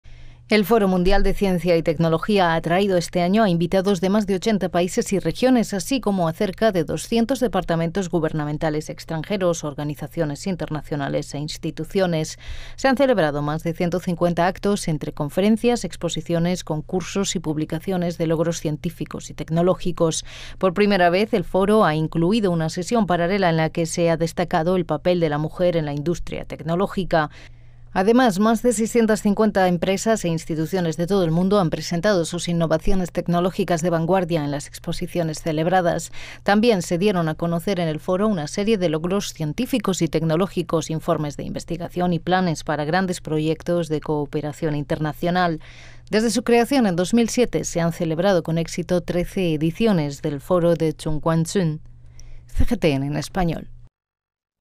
Se clausura en Beijing el foro de ciencia y tecnología
El 30 de mayo se clausura en Beijing el Foro de Zhongguancun, tras seis días de intensas jornadas. El foro mundial de ciencia y tecnología ha atraído este año a invitados de más de 80 países y regiones, así como a cerca de 200 departamentos gubernamentales extranjeros, organizaciones internacionales e instituciones.